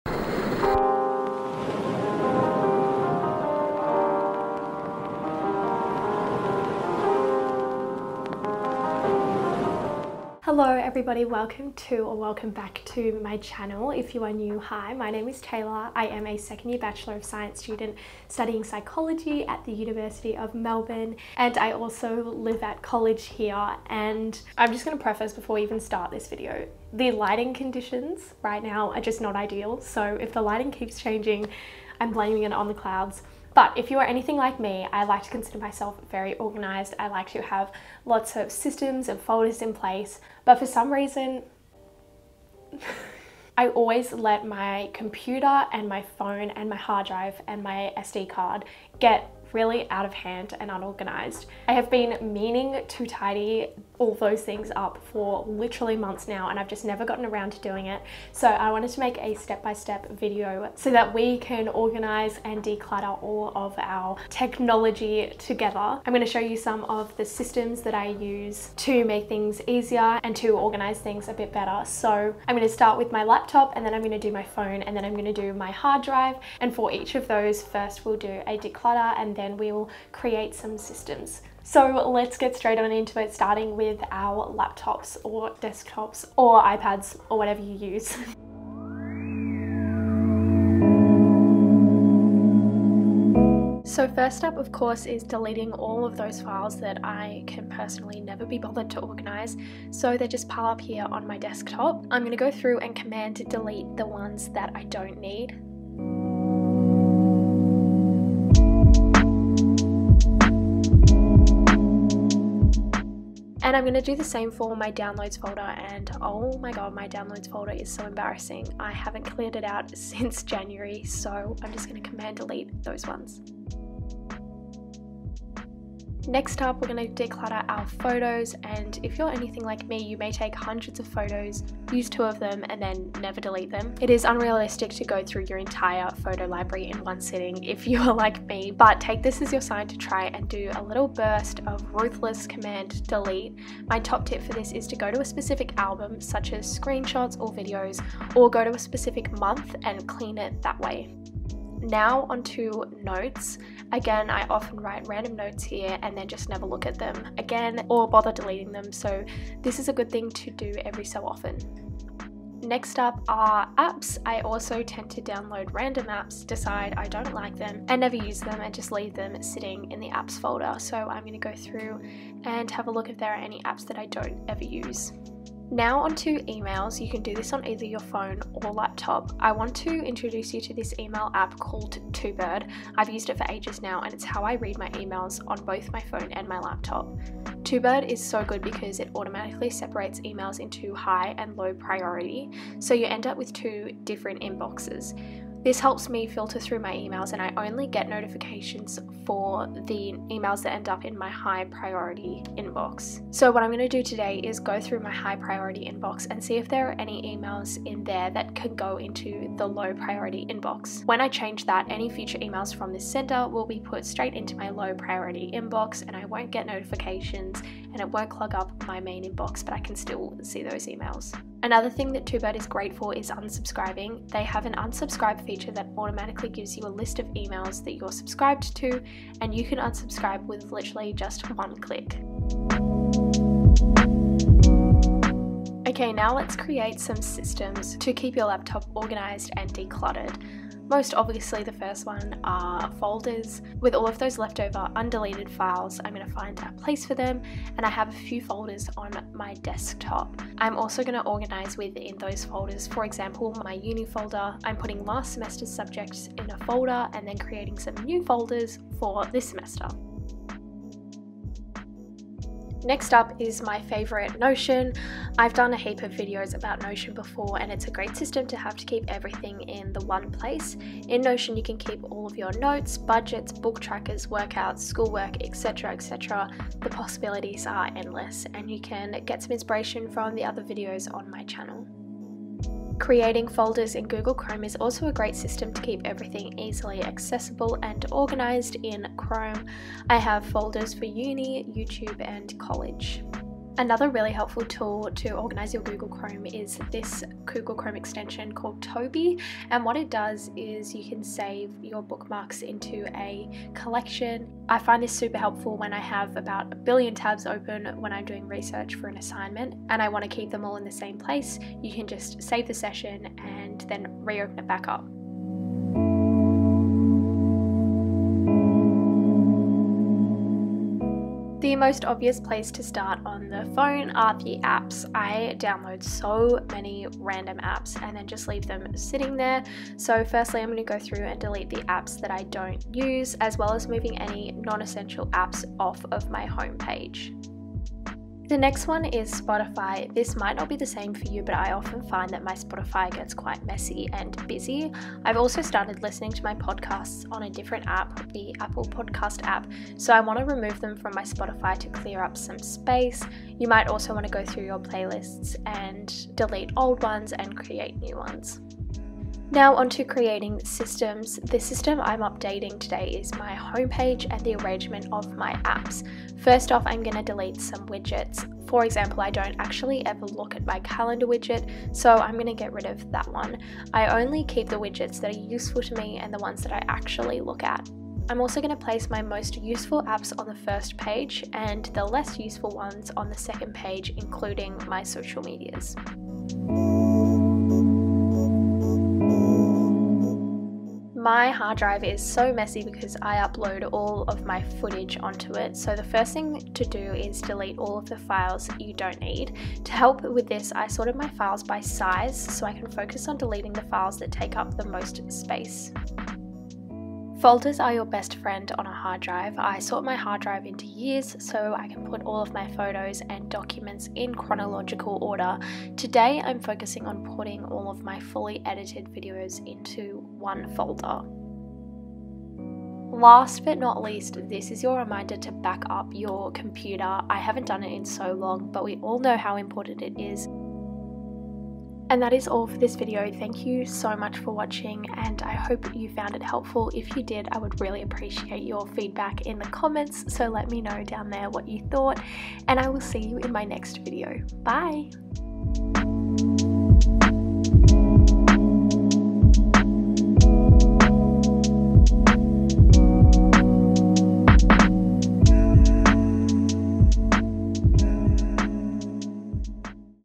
DIGITAL DECLUTTER | how to organise your laptop, phone and hard drive✨
In this video I take you through how to declutter your laptop/desktop, phone and hard drive step-by-step, first my decluttering and then by creating systems.

0:00 - introduction
1:28 - laptop
5:16 - phone
6:27 - hard drive

🤍 sub count
3130

about me
age: 19 (at time of upload)
studying: bachelor of science majoring in psychology
living: in melbourne, aus